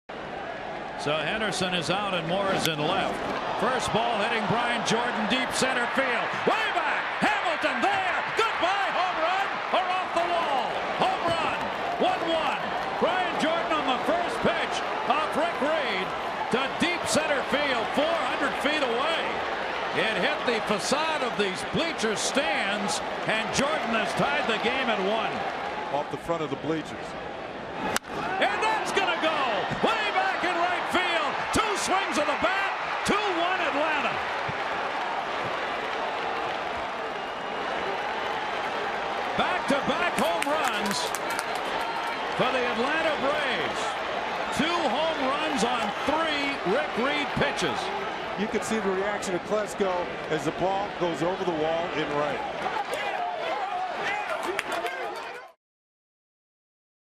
1999NLCS Gm4: Braves hit back-to-back homers for lead
10/16/99: Brian Jordan and Ryan Klesko hit back-to-back home runs in the 8th inning, giving the Braves a 2-1 lead over the Mets